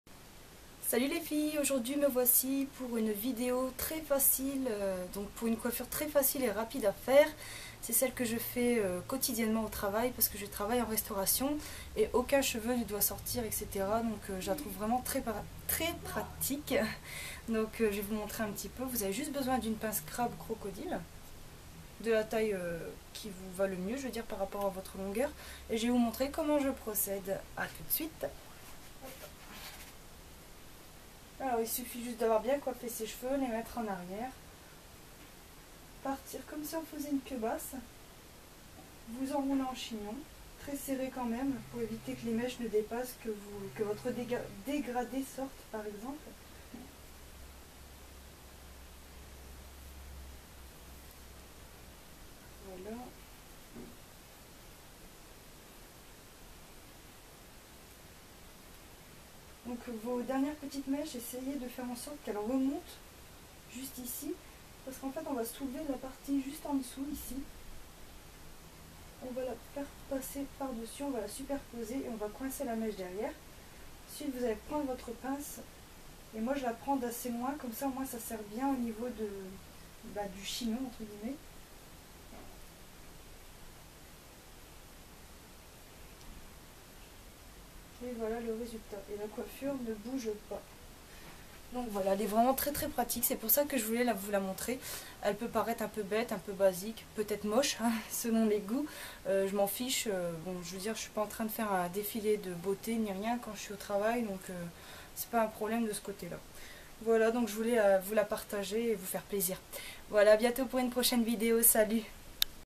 ✿⊱╮Coiffure facile & rapide qui donne des ondulations
Voici une coiffure facile, rapide et qui retient très bien les mèches de dégradés qui ont tendances à partir au fur et à mesure. Avec celle-ci je suis tranquille pendant des heures et quand je détache mes cheveux je finis avec de chouettes ondulations !